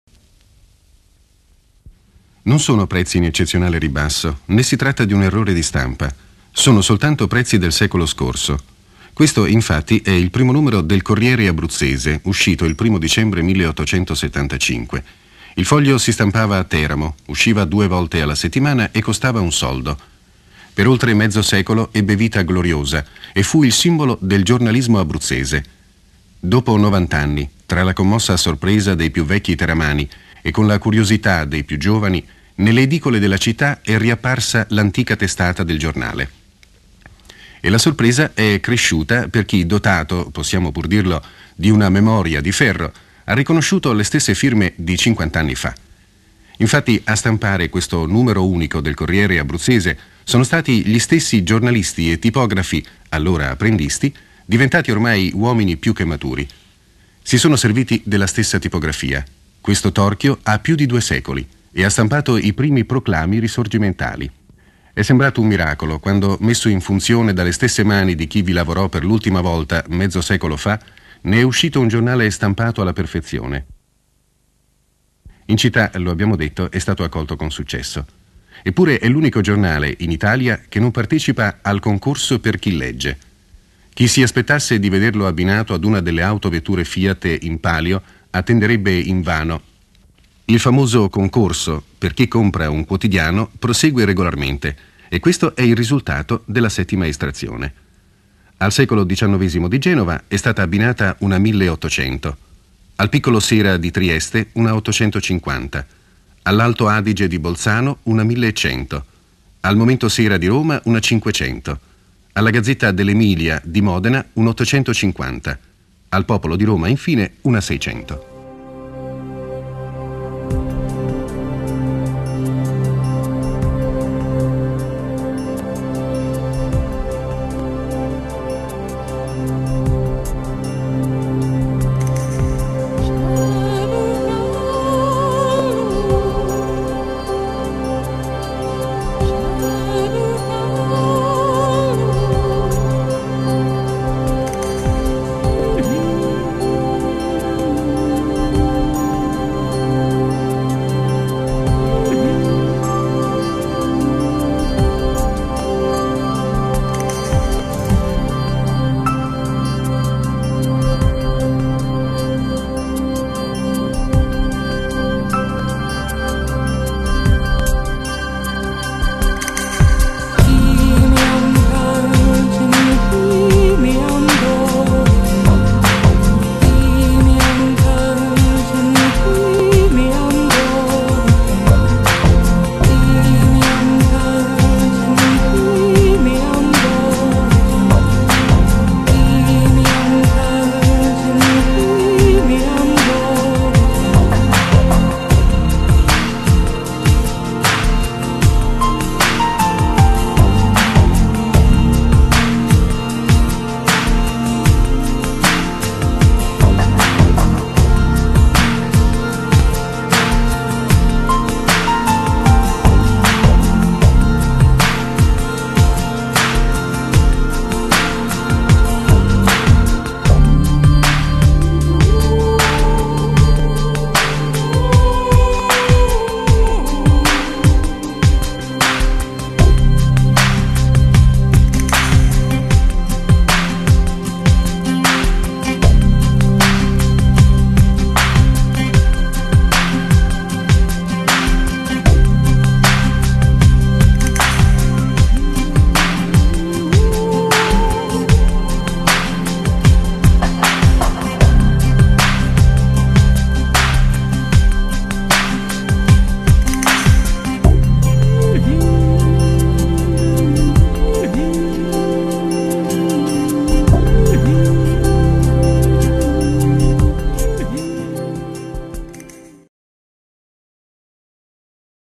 269 - Teramo negli anni 60
Il video è stato realizzato presumibilmente negli 1965 e documenta il ritorno in edicola di un quotidiano abruzzese, il Corriere Abruzzese, stampato per la prima volta nel 1875.

Il video si conclude con immagini della inagurazione della ex Villeroy & Boch.

Qui di seguito il testo del servizio giornalistico.

Non sono prezzi in eccezionale ribasso e ne si tratta di un errore di stampa.

Sono solo prezzi del secolo scorso.

Questo è infatti il primo numero del Corriere Abruzzese uscito il primo dicembre del 1875.

Il foglio si stampava a Teramo, usciva due volte la settimana e costava un soldo.

Per oltre mezzo secolo ebbe vita gloriosa e fu il simbolo del giornalismo abruzzese.

Dopo 90 anni, tra la commossa sorpresa dei più vecchi teramani e con la curiosità dei più giovani nelle edicole della città è riapparsa l'antica testata del giornale e la sorpresa è cresciuta per chi dotato, possiamo pur dirlo, di una memoria di ferro, ha riconosciuto le stesse firme di 50 anni fa.

Infatti a stampare questo numero unico del Corriere Abruzzese sono stati gli stessi giornalisti e tipografi, allora apprendisti, diventati uomini più che maturi.

Si sono serviti della stessa tipografia e questo torchio ha più di due secoli e ha stampato i primi proclami risorgimentali.

E' sembrato un miracolo quando messo in funzione dalle stesse mani di chi vi lavorò per l'ultima volta mezzo secolo fa ne è uscito un giornale stampato alla perfezione.

In città, l'abbiamo detto è stato accolto con successo.

Eppure è l'unico giornale, in Italia, che non partecipa al concorso "Per chi legge".

Chi si aspettasse di vederlo abbinato ad una delle autovetture Fiat in palio attenderebbe invano.

Il famoso concorso per chi compra un quotidiano prosegue regolarmente e questo è il risultato della settima estrazione.

Al Secolo XIX di Genova è stata abbinata una 1800, al Piccolo Sera di Trieste una 850, all'Ato Adige di Bolzano una 1100, al Momento Sera di Roma una 500, alla Gazzetta dell'Emilia di Modena una 850, al Popolo di Roma, infine, una 600